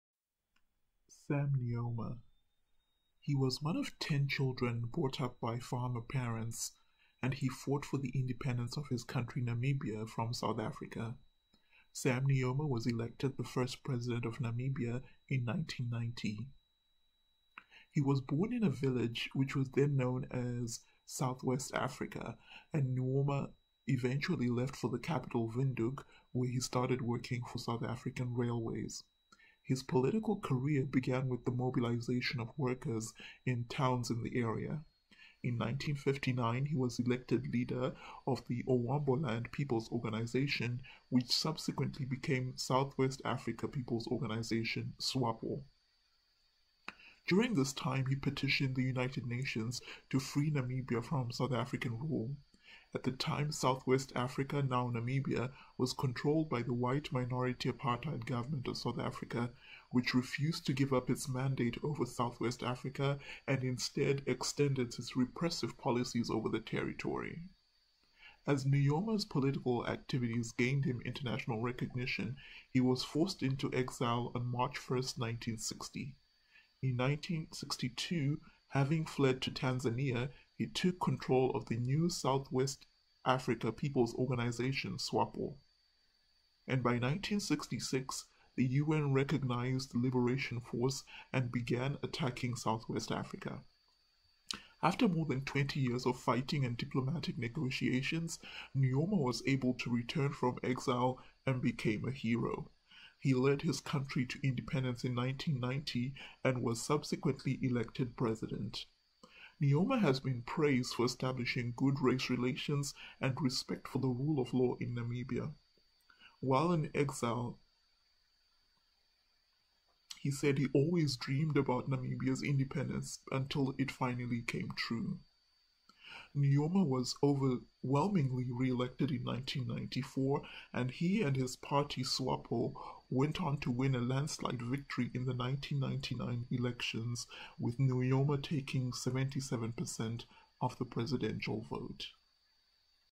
Sam Nujoma: First President of Namibia
One of 10 children brought up by farmers, Sam Nujoma fought for the independence of his country from South Africa and was elected the first president of the new nation, Namibia, in 1990.